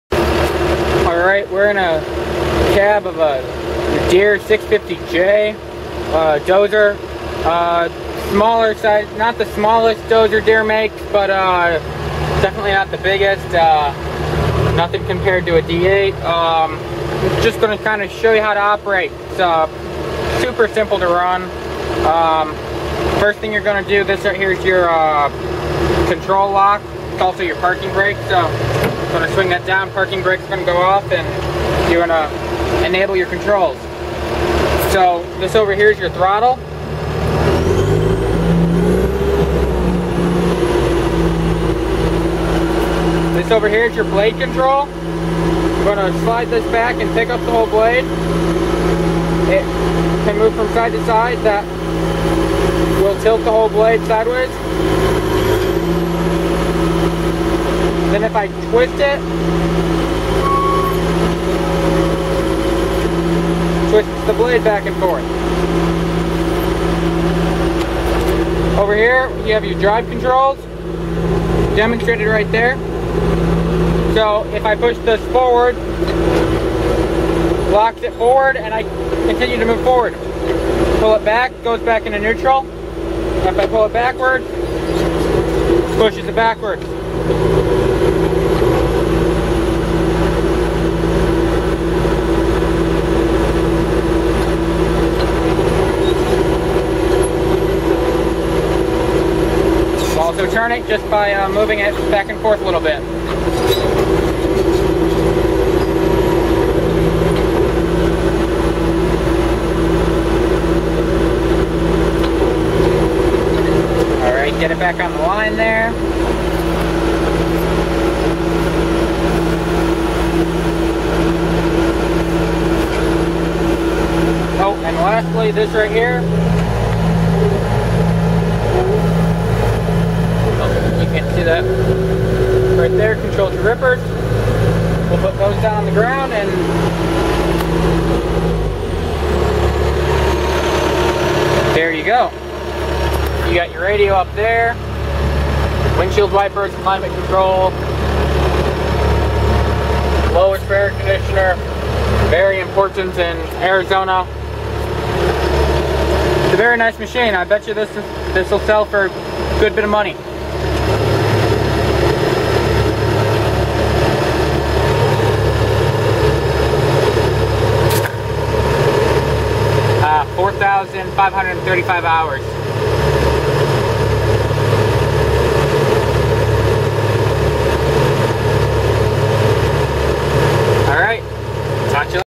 Deere 650J Dozer: How to Operate
A really basic how to on a Deere 650J, one thing that I forgot to mention that is kind of important (never crossed my mind being in the Ritchie Bros. yard) is the gear shifter. It is the grey buttons on the steering joystick.